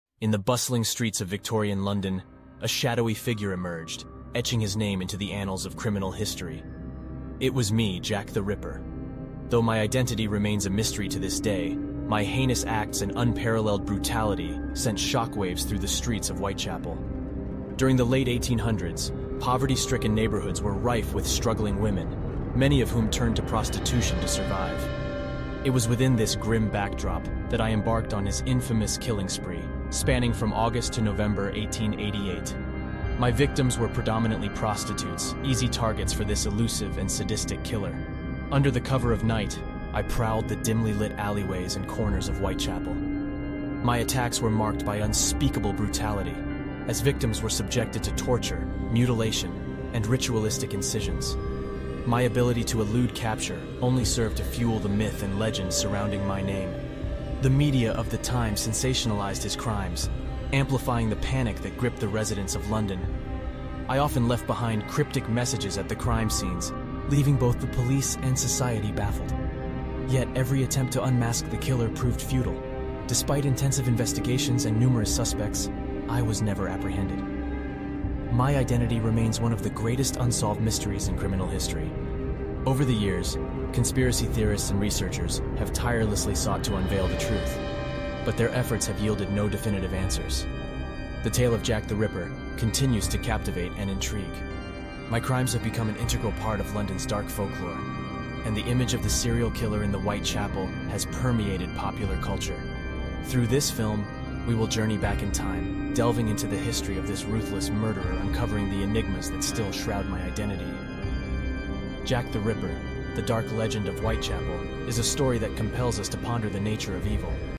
Unmasking Jack the Ripper: The Dark Secrets of Whitechapel
It  is a gripping documentary that delves into the enigmatic case of one of history's most notorious serial killers. Join us as we peel back the layers of mystery surrounding Jack the Ripper, exploring the dark alleys of Victorian London's Whitechapel district. Through meticulous research, expert analysis, and chilling reenactments, we aim to uncover the truth behind the elusive killer's identity and shed light on the haunting legacy that continues to captivate the world. Prepare to be immersed in a chilling journey into the heart of one of history's most enduring enigmas.